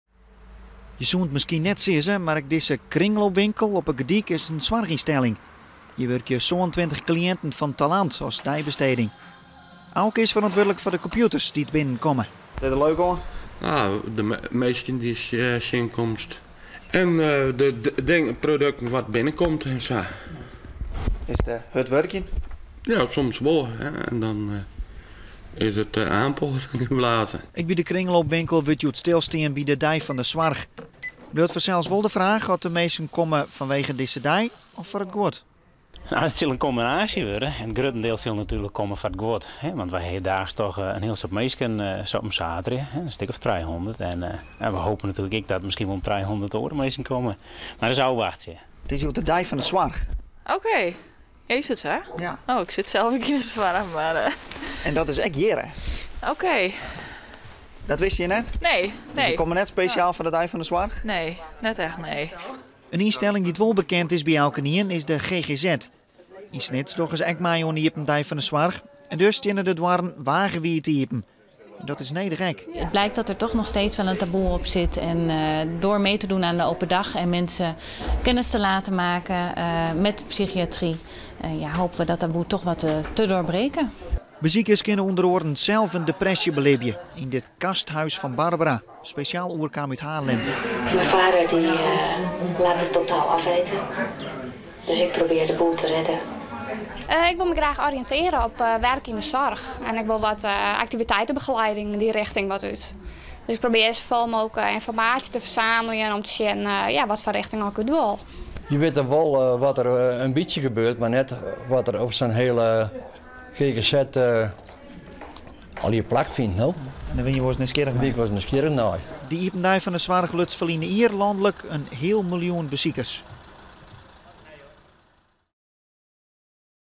Tûzenen op iepen dei fan de soarch
Santich soarchynstellings yn Fryslân hâlde sneon iepen dei. It is dan de lanlike aksjedei 'Kom kijken in het hart van de zorg'.

Guon ynstellings meitsje in soad wurk fan de iepen dei. Sa hat bygelyks soarchsintrum Sint Theresia op 'e Jouwer in grut feest mei âld-Hollânske spultsjes. By de GGZ-ynstelling yn Snits is it mooglik yn in 'kasthûs' in film oer depresjes te sjen. Sa kin it publyk sels ûnderfine wat dat is. De kringloopwinkel op de Gordyk die ek mei.

Zeventig zorginstellingen in Fryslân houden zaterdag open huis. De openstelling vindt plaats in het kader van de landelijk actiedag 'Kom kijken in de zorg'.

Sommige instelling maken veel werk van de open dag. Zo heeft zorgcentrum Sint Theresia in Joure een feest georganiseerd met oud-Hollandse spelletjes. Bij de GZZ in Sneek konden mensen in een 'kasthuys' een film over depressies zien. Zo kan het publiek zelf ervaren hoe dat is. De kringloopwinkel in Gorredijk deed ook mee aan de open dag.